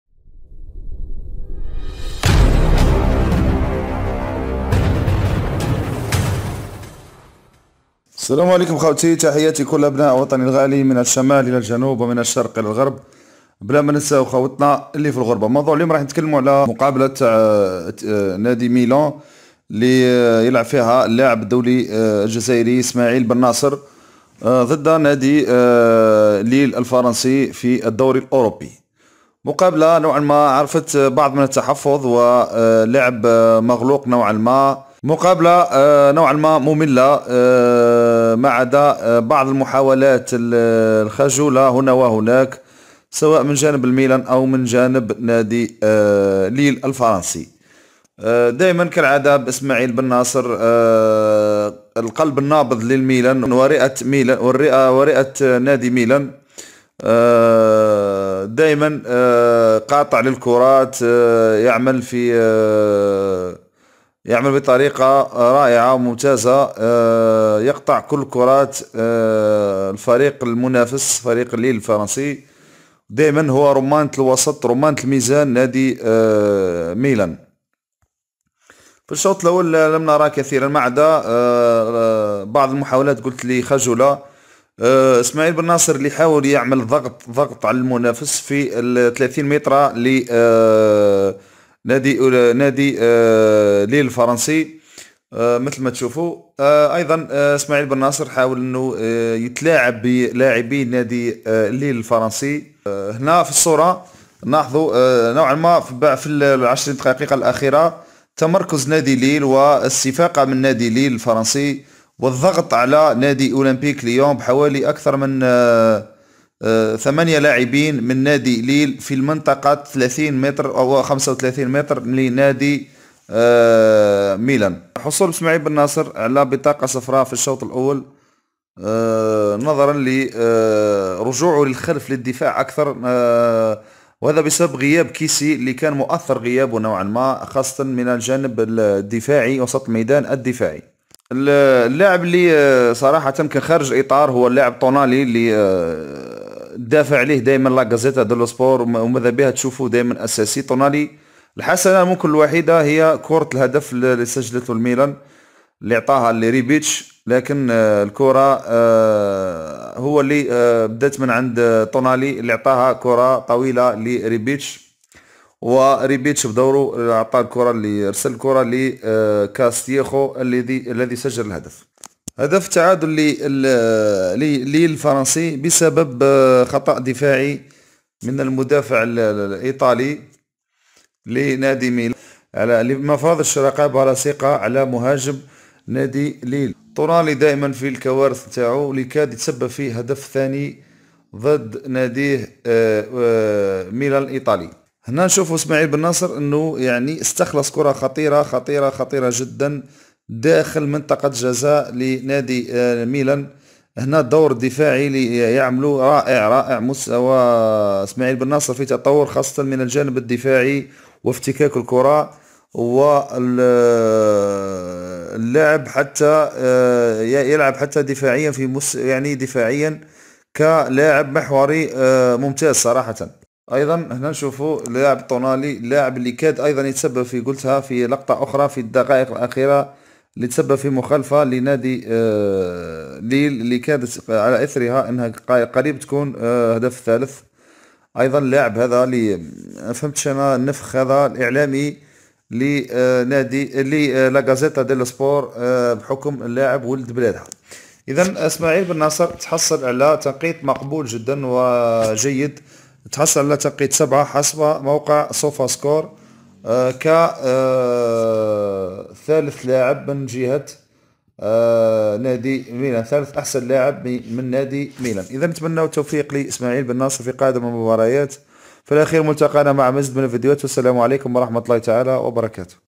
منقذ الميلان هو المايسترو
بن ناصر يواصل عروضه القوية كأساسي في الميلان
لمزيد الفيديوهات لاتنسوا الإشتراك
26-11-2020